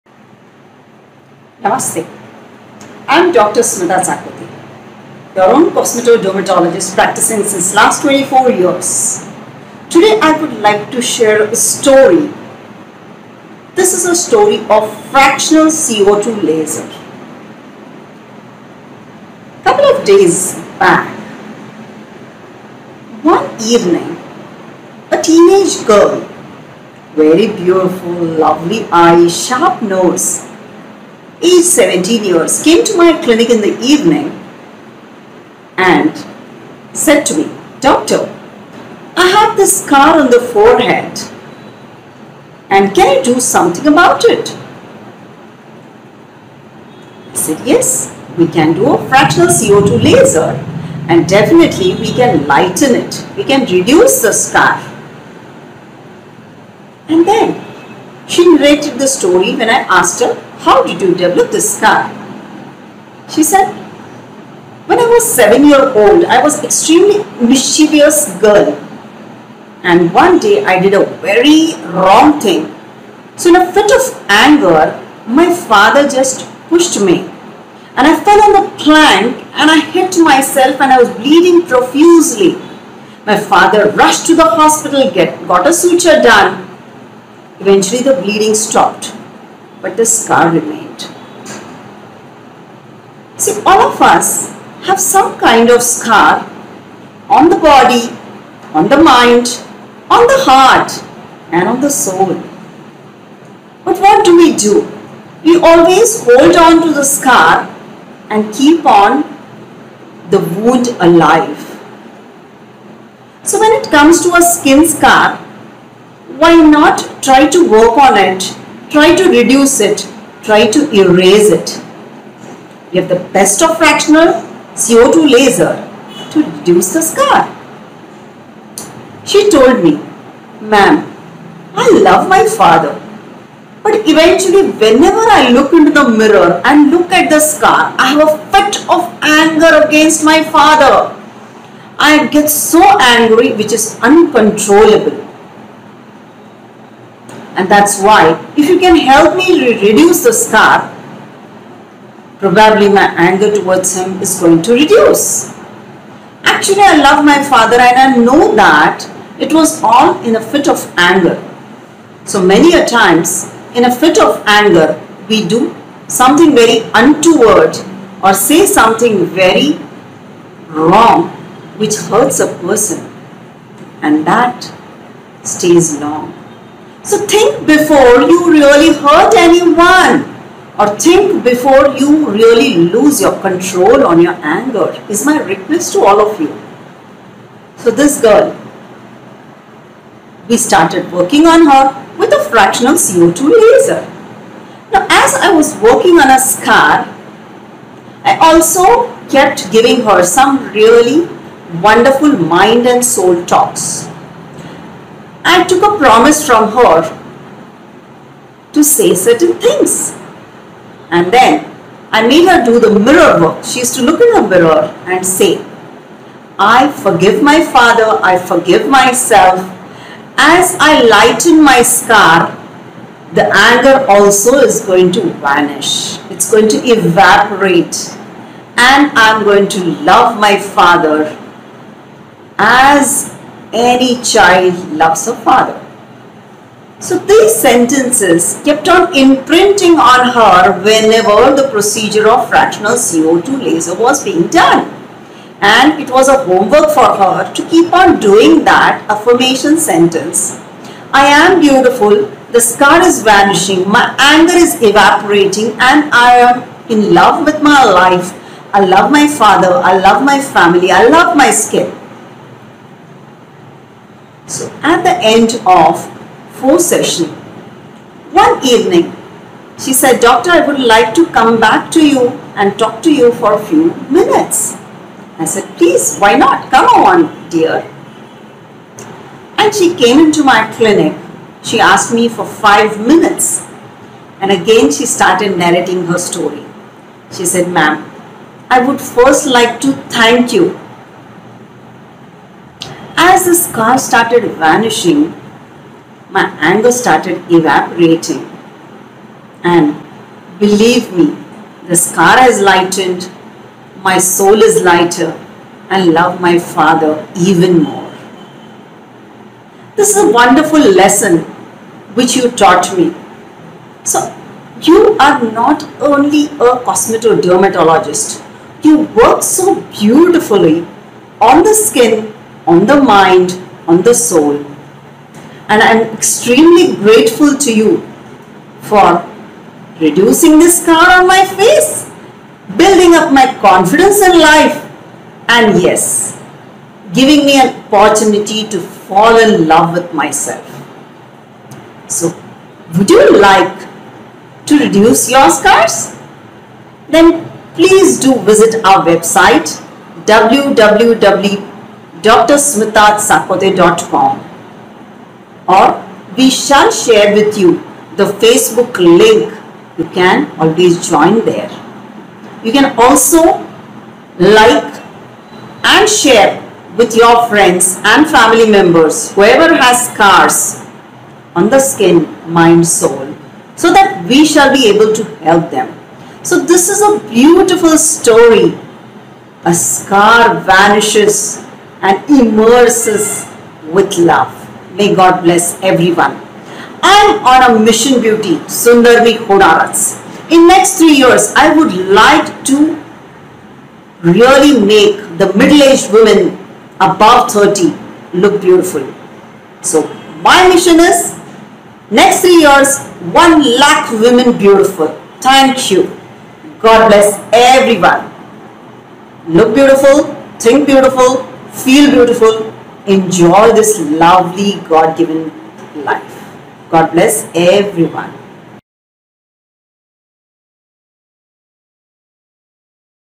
Mission Beauty सुंदर मी होणारच 23- A True story of scar(Makeover Journey) - Fractional CO2 Laser
makeovers from ugly to beautiful affirmations for beautiful face co2 laser treatment for scars story of father and daughter relationship
This is a True story of  a Young Girl with a scar on her face given by her Father's anger with Laser we reduce the scar with Doctor's Motivation and affirmation anger evaporated.
 10 years of anger evaporated and a real father to daughter love relationship established. A True Makeover journey .